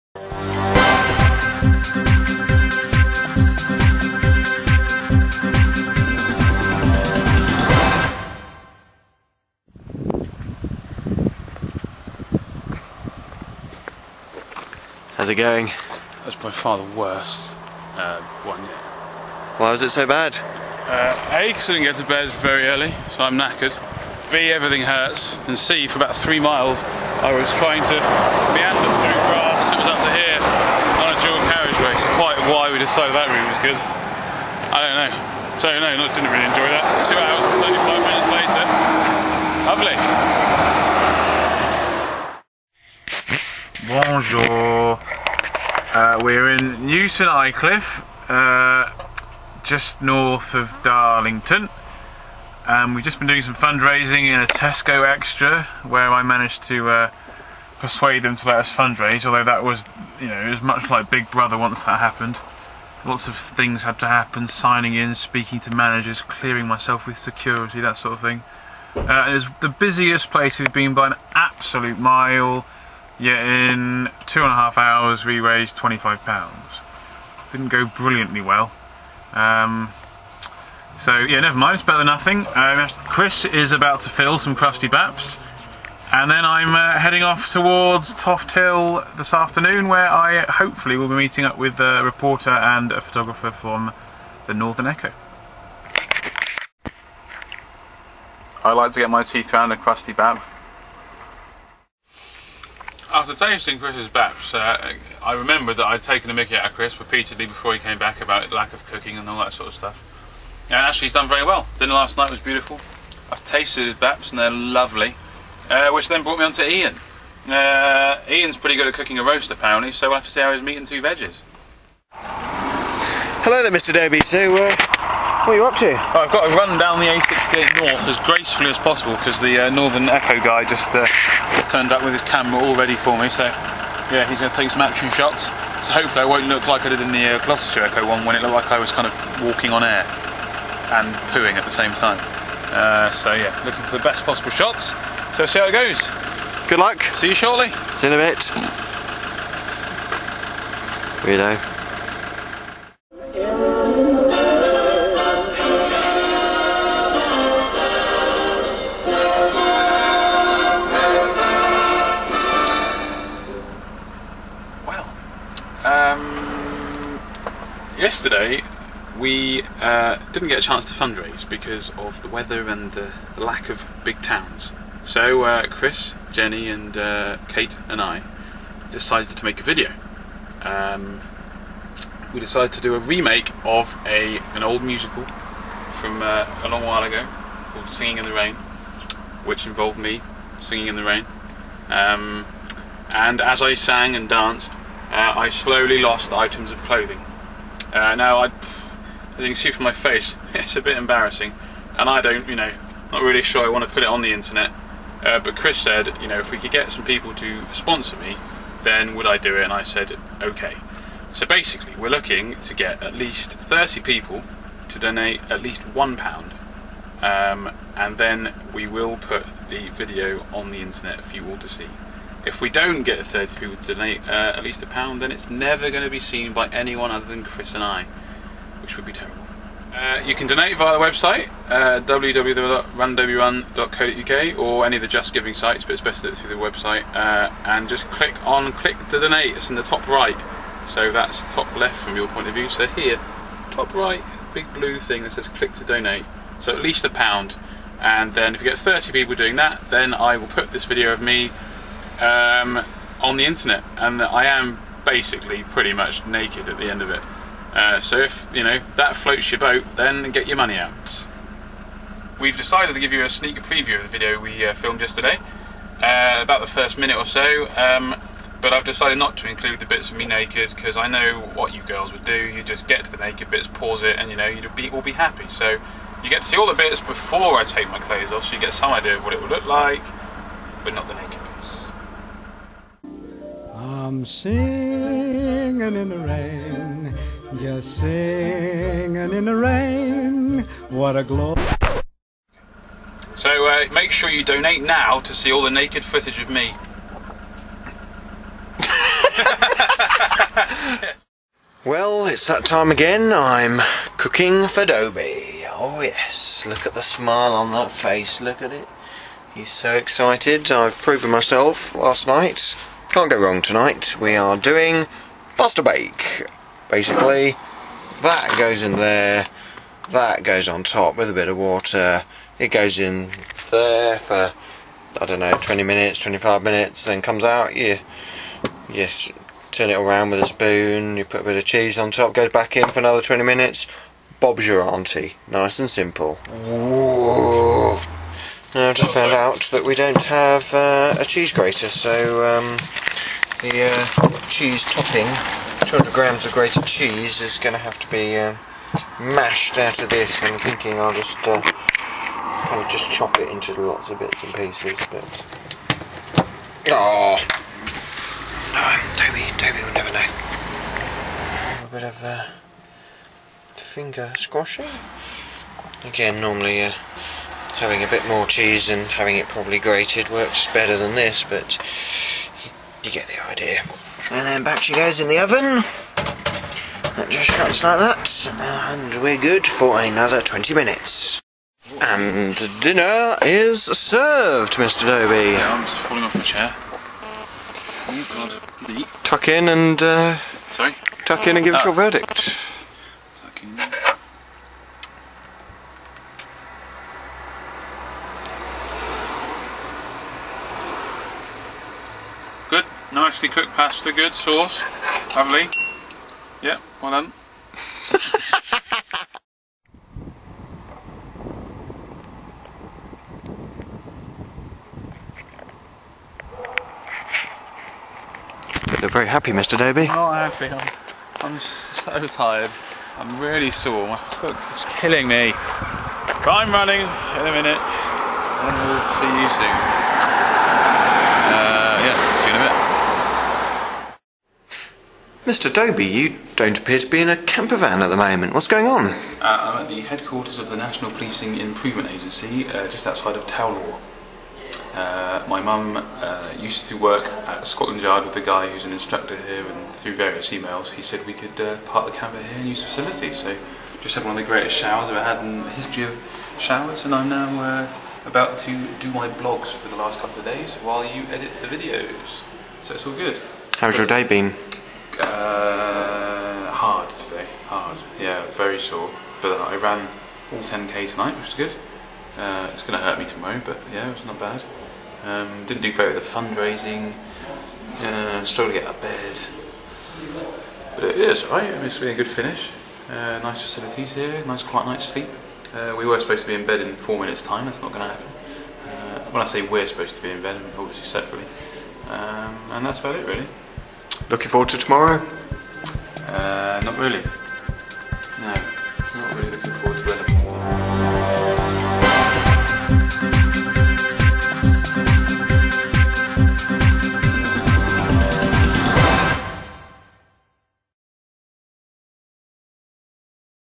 Run Dobie Run - Day 16 - Tuesday 25th August 2009
A late night meant a tough day for Andrew.  Cook along with Chris returns after popular demand and Andrew gives a sneak preview to his antics on Monday - get donating people!